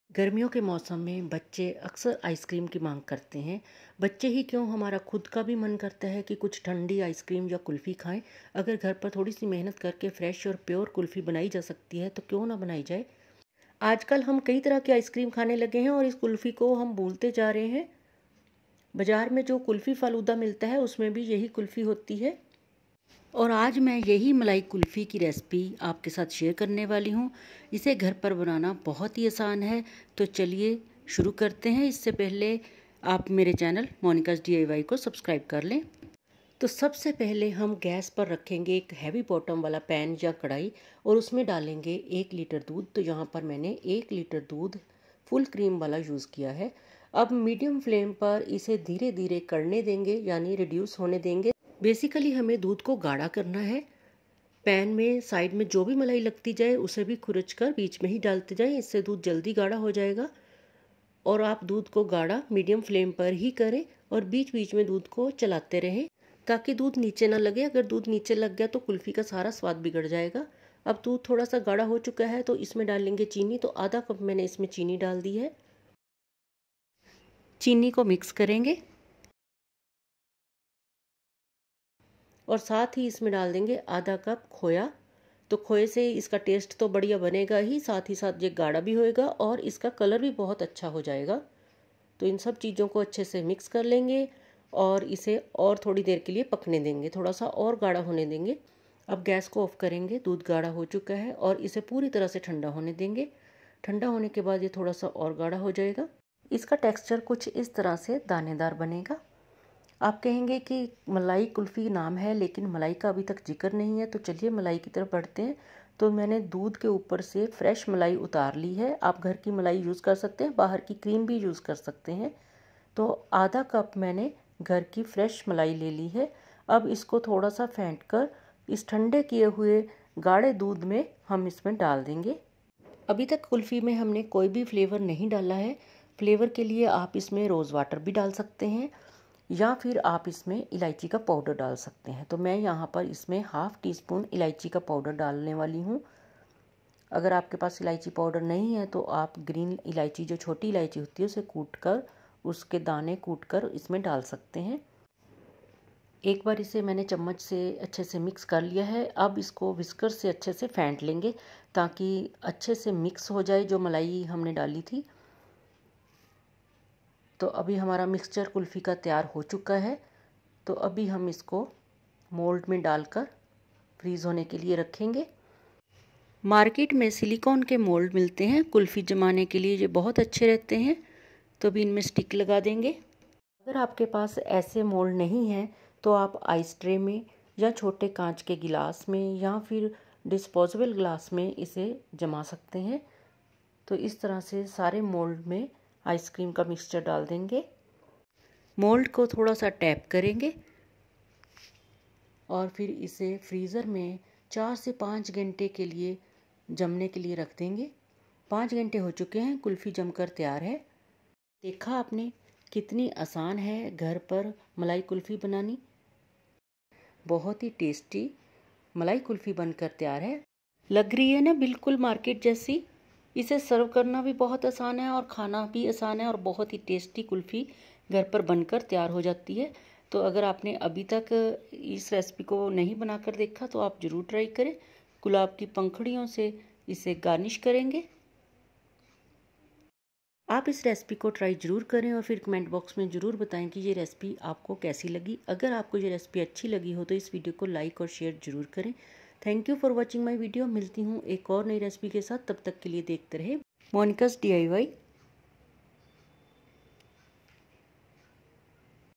मलाई कुल्फी घर पर बनाने का आसान तरीका | How to Make Malai Kulfi at Home – Monikazz DIY
If you liked this videos of ... then please Like it, Share It.